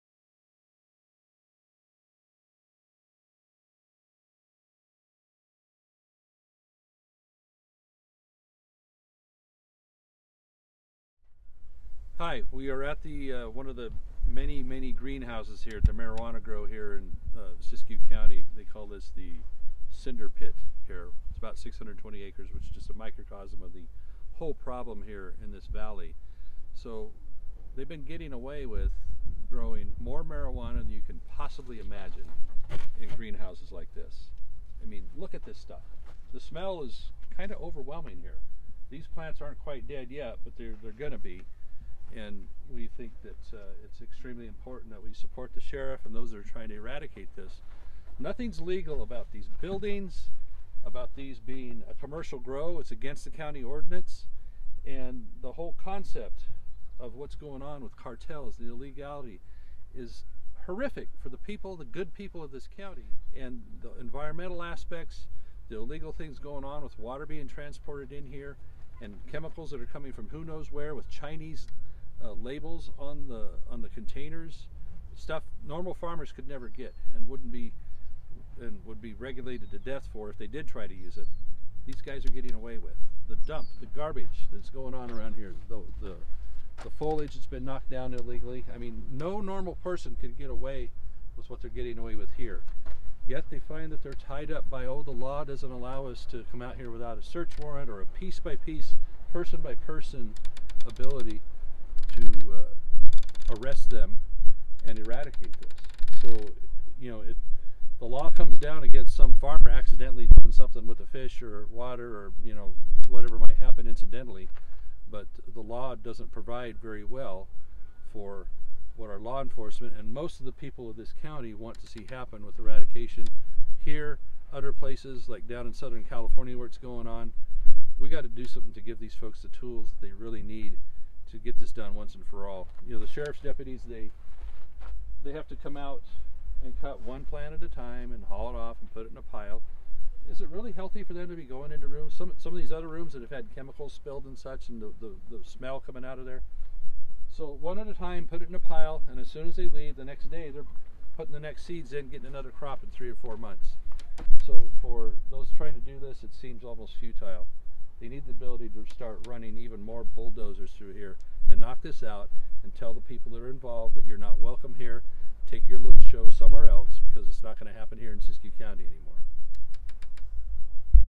Rep. LaMalfa exposing the pollution at the Illegal cartel grow site
Nothing about what is happening at the grow sites in Siskiyou county is legal. Working together we have to fix this problem.